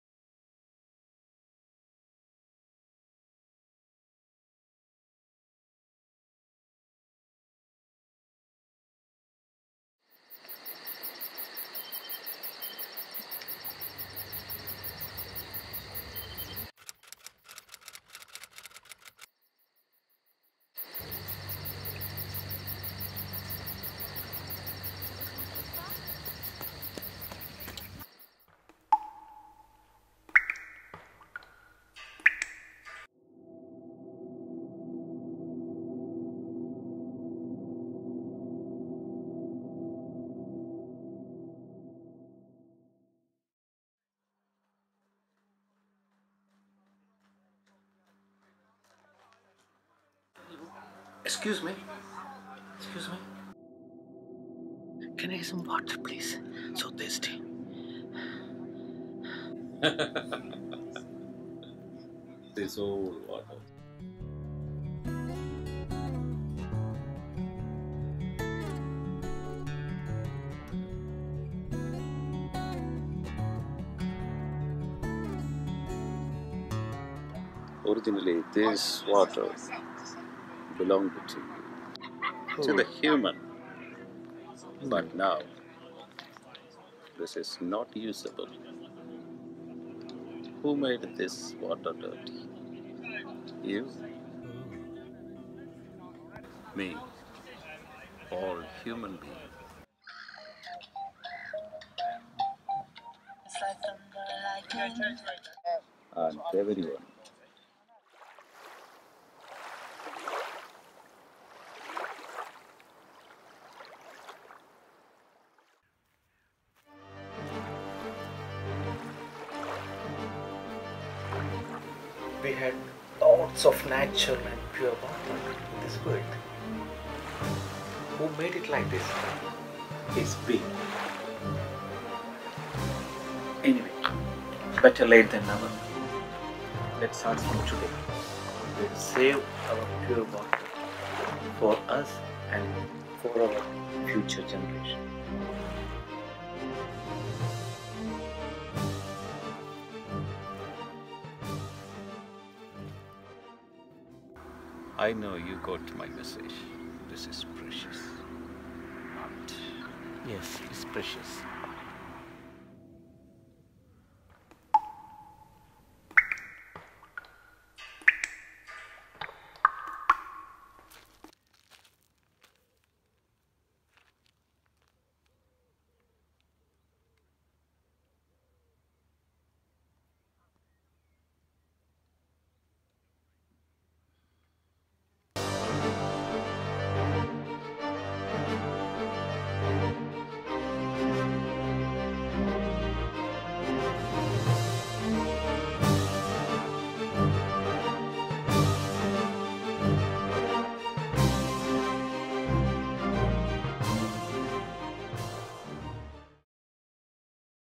Water Precious
A short film by Rotary club Perinthalmanna Kerala, India.  Shooted in Cambridge UK. All videos are shooted in iPhone 7 Plus